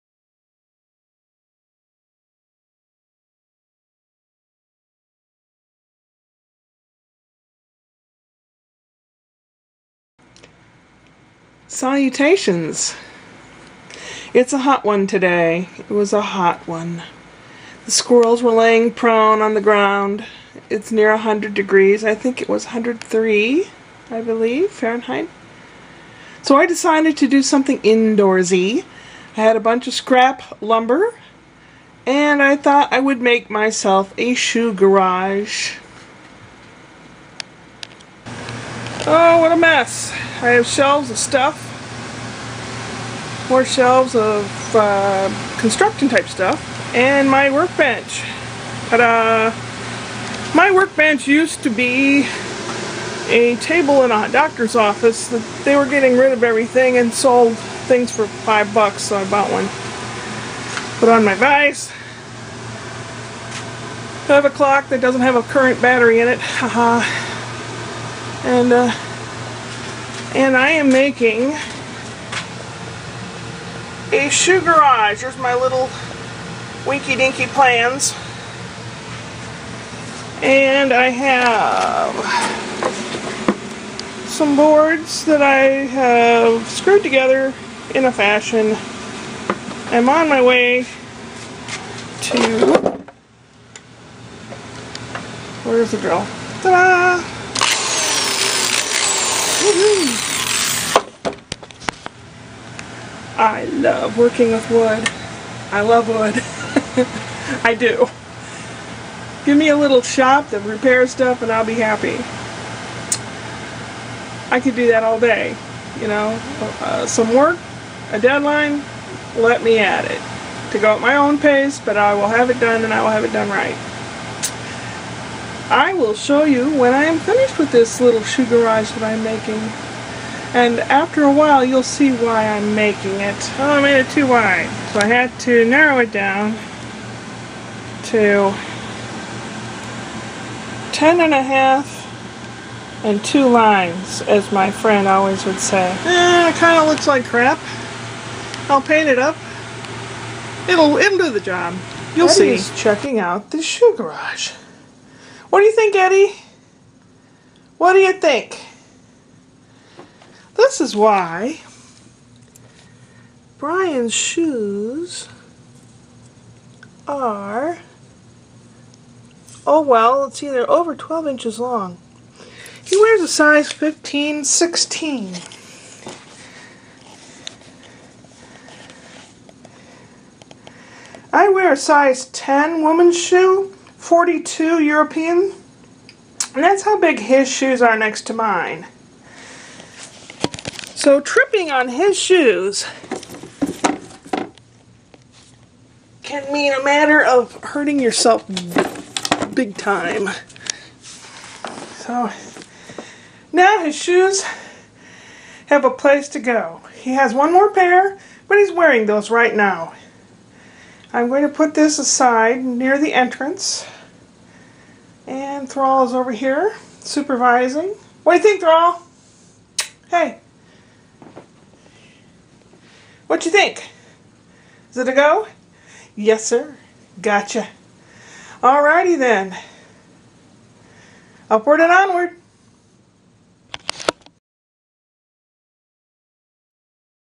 YTO 106 Shoe Garage
too hot to go out..so I took some wood odds and ends that I had, and built a "shoe garage".. for Brian's shoes. He wears a size 16 men..I don't know what number that is european.. I wear an 8.5 american men(10women)which is size 42 European.
I trip over them often and decided that the shoes needed a home. Today, it was 103Fahrenheit..eee gad zooks.